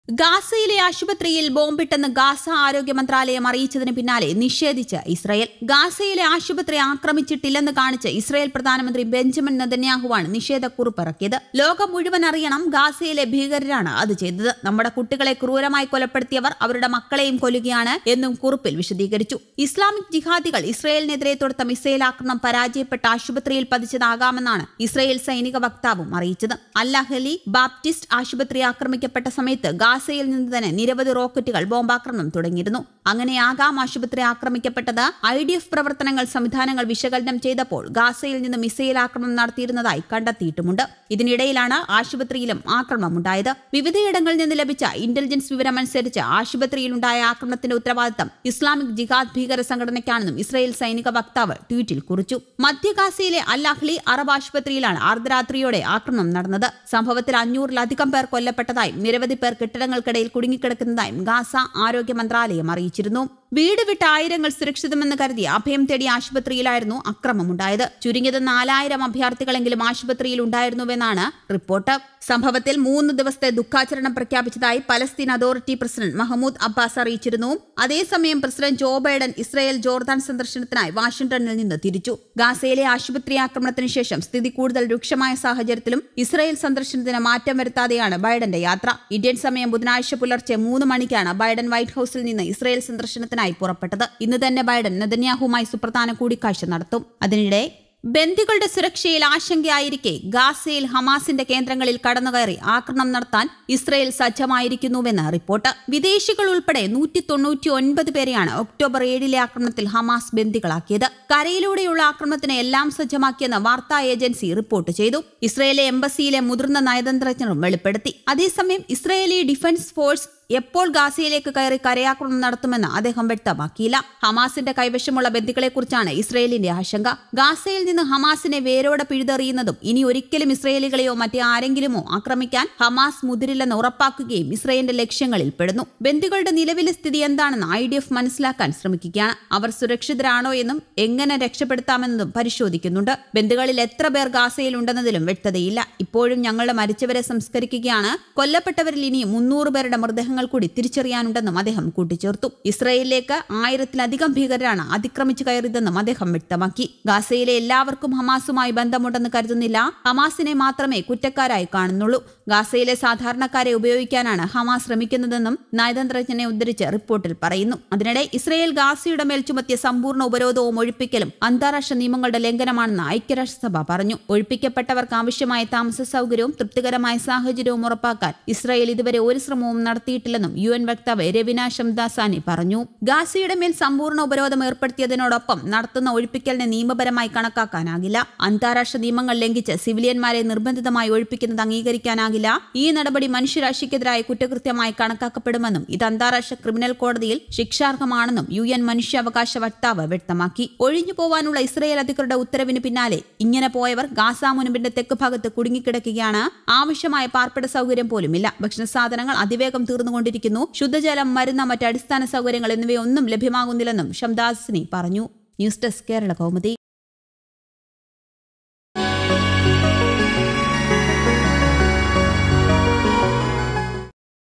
ഗാസ ആശുപത്രിയില്‍ ബോംബ് വര്‍ഷം, നിലവിളി, നിഷേധിച്ച് ഇസ്രയേല്‍ | Israel War News
ഗാസയിലെ ആശുപത്രിയില്‍ ബോംബിട്ടെന്ന് ഗാസ ആരോഗ്യമന്ത്രാലയം അറിയിച്ചതിന് പിന്നാലെ നിഷേധിച്ച് ഇസ്രയേല്‍. ഗാസയിലെ ആശുപത്രി ആക്രമിച്ചിട്ടില്ലെന്ന് കാണിച്ച് ഇസ്രായേല്‍ പ്രധാനമന്ത്രി ബെഞ്ചമിന്‍ നെതന്യാഹുവാണ് നിഷേധ കുറിപ്പിറക്കിയത്. ലോകം മുഴുവന്‍ അറിയണം. ഗാസയിലെ ഭീകരരാണ് അത് ചെയ്തത്. നമ്മുടെ കുട്ടികളെ ക്രൂരമായി കൊലപ്പെടുത്തിയവര്‍ അവരുടെ മക്കളെയും കൊല്ലുകയാണ്. എന്നും കുറിപ്പില്‍ വിശദീകരിച്ചുന്നു.ഇസ്ലാമിക് ജിഹാദികള്‍ ഇസ്രയേലിനെതിരെ തൊടുത്ത മിസൈല്‍ ആക്രമണം പരാജയപ്പെട്ട് ആശുപത്രിയില്‍ പതിച്ചതാകാമെന്ന് ഇസ്രായേല്‍ സൈനിക വക്താവും അറിയിച്ചു അല്‍ അഹ്ലി ബാ്ര്രപിസ്റ്റ് ആശുപത്രി ആക്രമിക്കപ്പെട്ട സമയത്ത് ഗാസയില്‍ നിന്ന് തന്നെ നിരവധി റോക്കറ്റുകള്‍ ബോംബാക്രമണം തുടങ്ങിയിരുന്നു. അങ്ങിനെയാകാം ആശുപത്രി ആക്രമിക്കപ്പെട്ടത്.